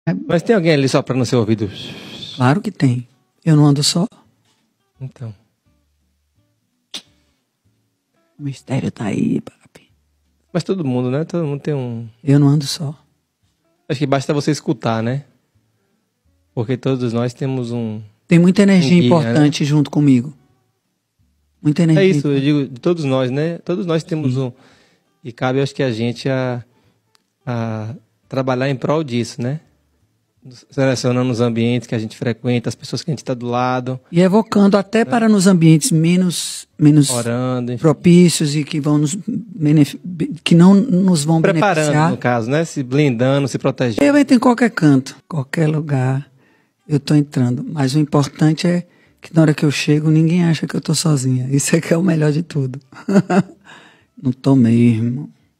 Claro que tem, eu nunca ando só - Ivete Sangalo e Daniel Cady
Esse trecho foi retirado do meu podcast, o Cady Cast e no link abaixo você pode assistir ao vídeo completo 👇

Se ainda não me segue nas outras redes é só procurar: @danielcady

Instagram da convidada: @ivetesangalo